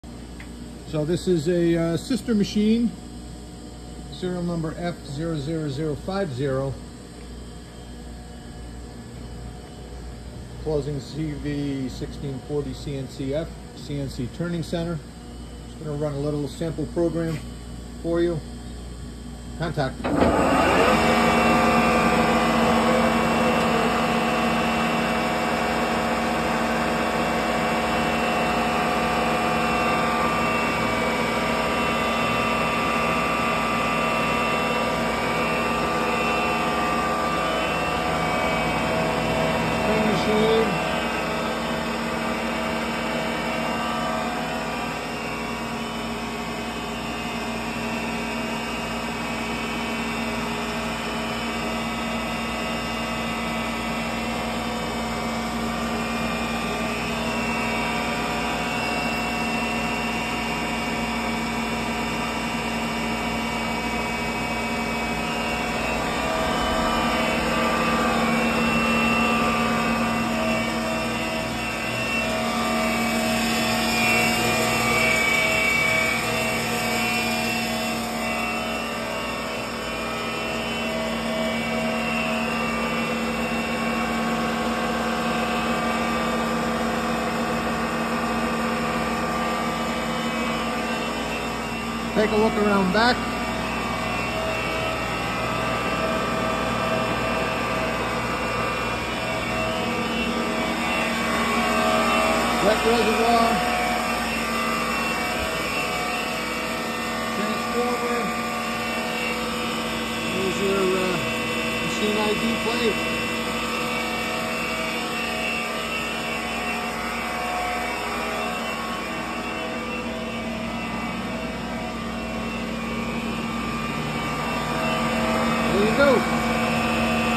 2007 Clausing CV1640CNCF CNC Turning Center up for auction!
2007 Clausing CV1640CNCF Model CKE6140E CNC Turning Center S/N F00051, Fagor Controls, Quick Draw Bar, Servo Powered Tool Post to be sold at auction May 15 via online auction. Inspection is May 13 & 14 at 89 Mill Brook Rd Plainfield CT. BUY RIGHT! BUY AT AUCTION!